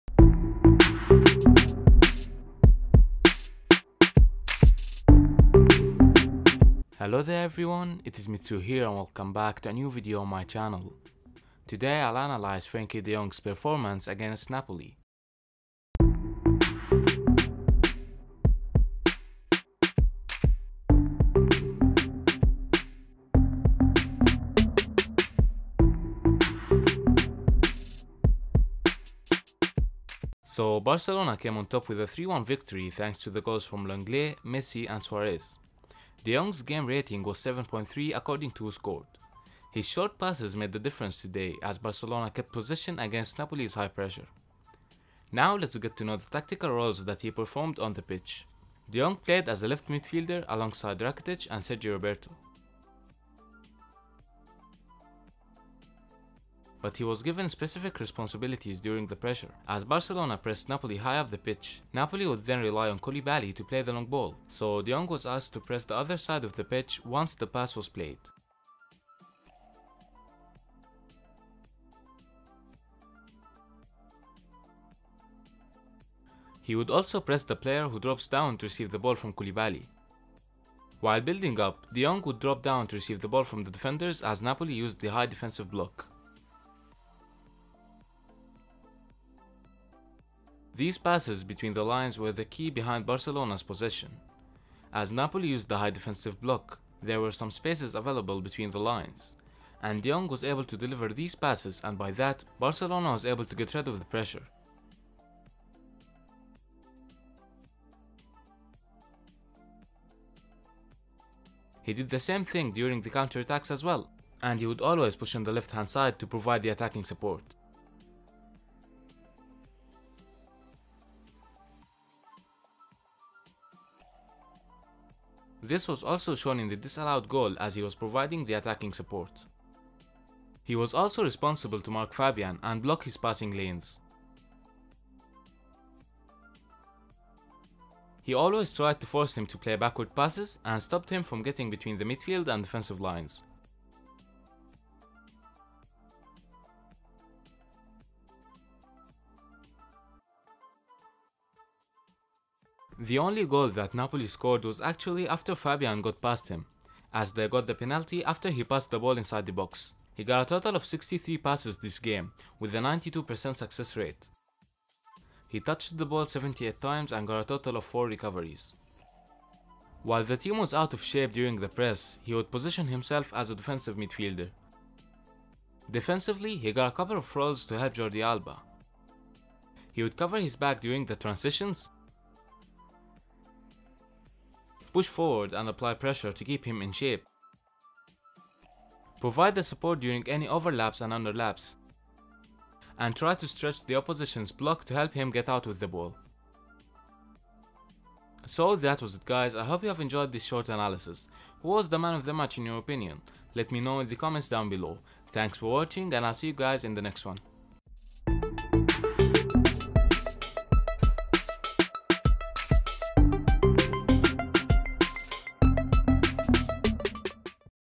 Frenkie De Jong VS Napoli / Player Analysis / What Was His Crucial Role In Barcelona's Possession?
In this video, I tried to analyze Frenkie De Jong's performance against Napoli. Barcelona got a 3-1 victory thanks to the goals of Lenglet and Lionel Messi and Luis Suarez. I've discussed the roles that were given to him offensively as well as defensively. Thanks.

Make sure to check out KLIPDRAW the editing software